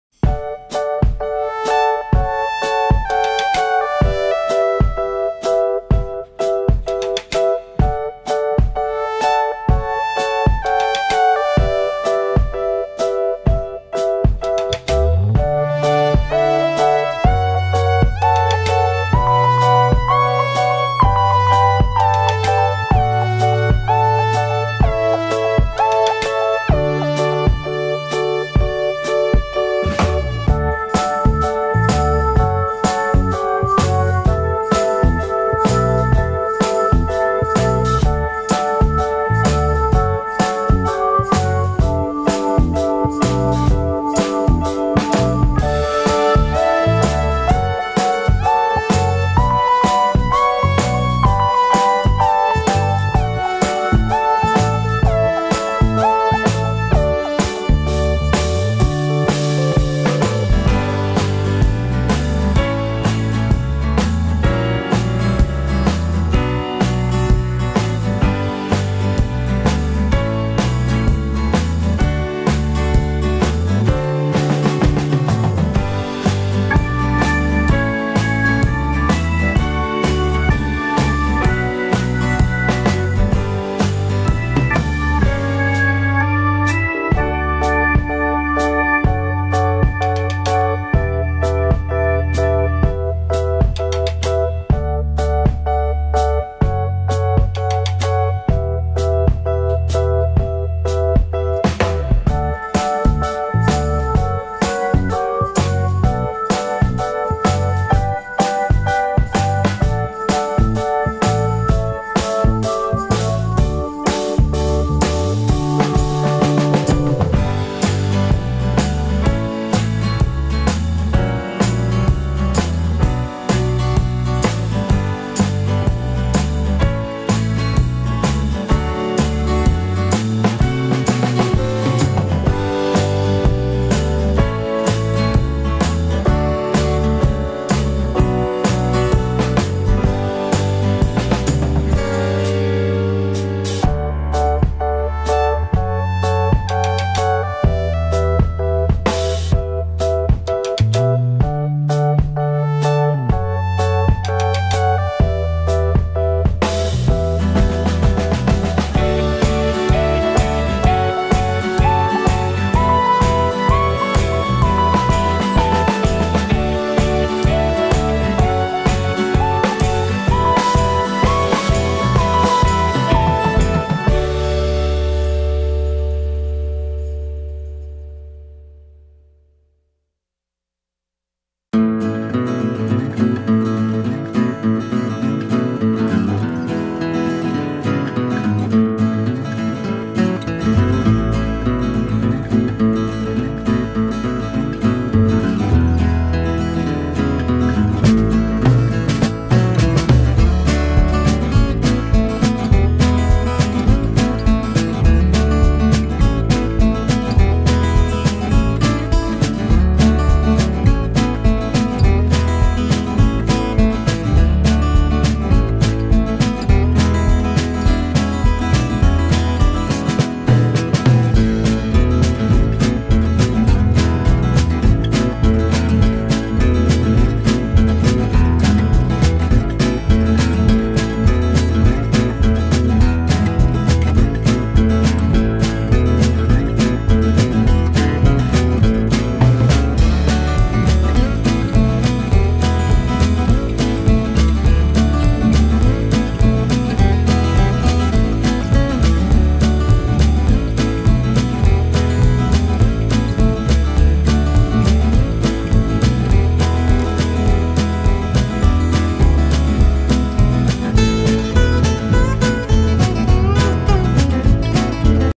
Squirtle evolves! Sculpting Wartortle Water-type Pokémon in Clay
Difficulty : ★★★★☆

Hello, I am Squash Clay.
In this video, I sculpted Wartortle figurine from Pokemon with super light clay. The clay I am using is not shipped overseas. I recommend searching for 'super light clay' at the AliExpress site. I wish my video help you make figurine.